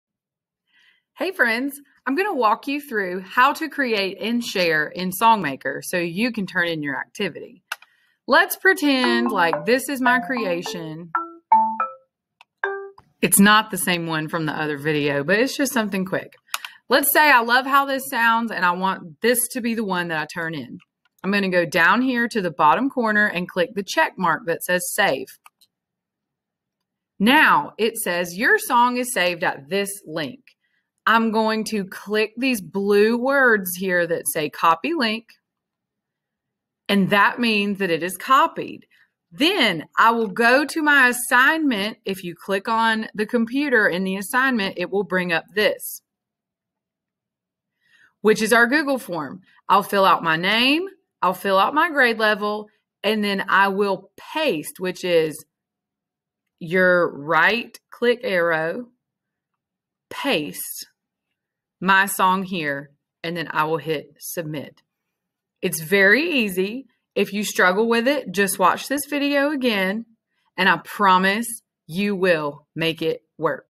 How to Share Song Maker Creation
Moment of Music with Mrs. Sloan
How to share your creation with me and complete your assignment.
Chrome Music Lab Song Maker